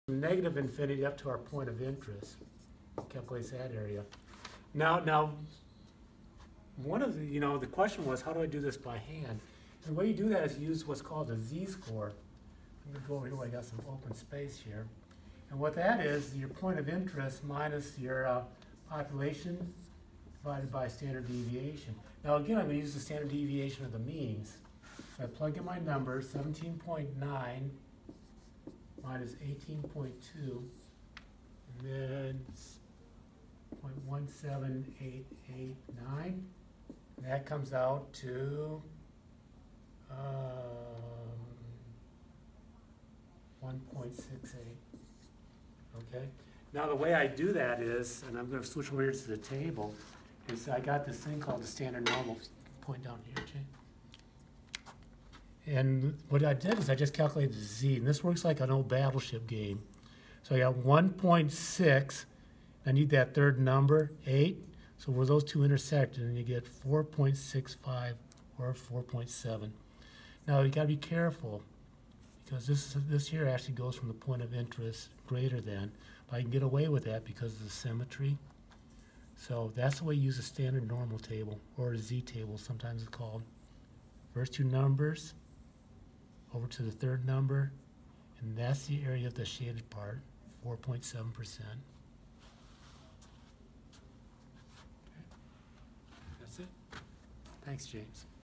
Advanced Six SIgma Greenbelt, Unit 2, Question 6
The amount of paint used on new tractors averages 18.2 gallons with a standard deviation of 0.8 gallons. A new nozzle is used on 20 tractors and averaged 17.9 gallons. What is the probability that is was just random chance?